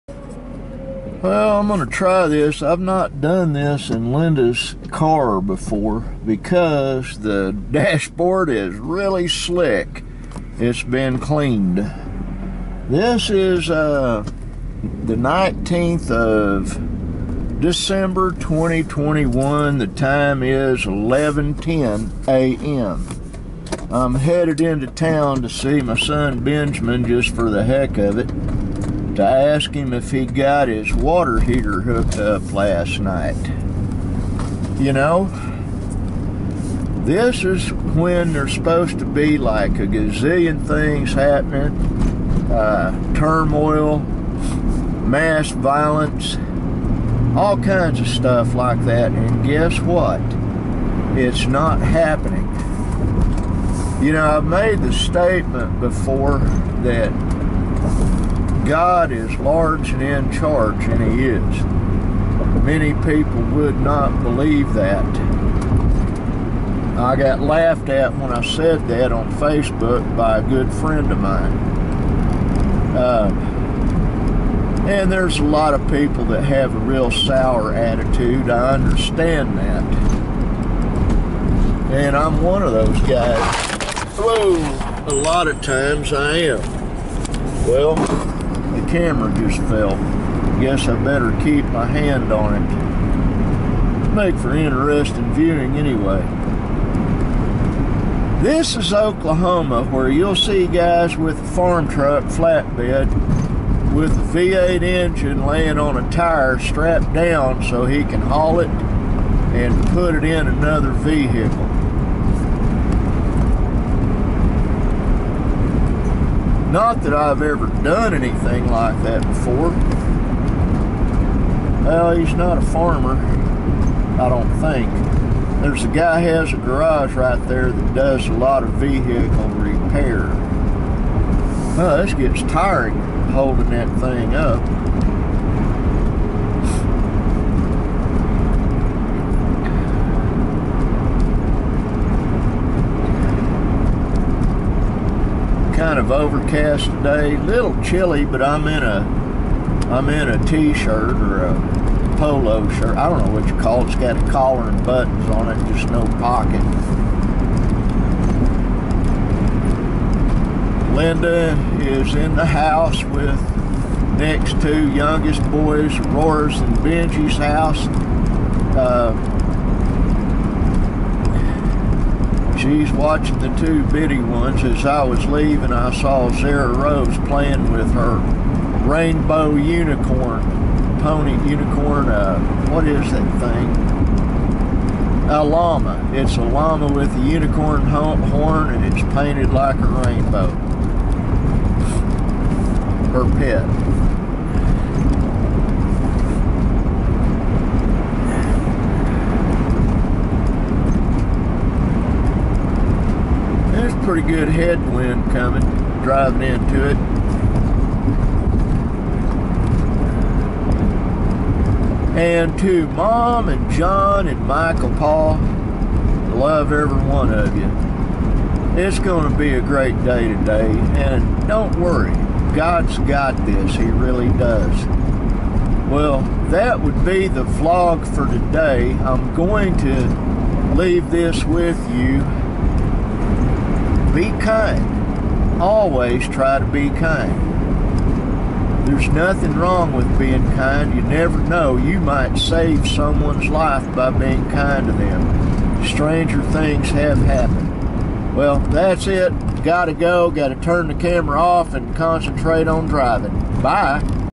My Tuesday Travels with Tuklo vlog.
My camera slides all over the dash of my wife"s car.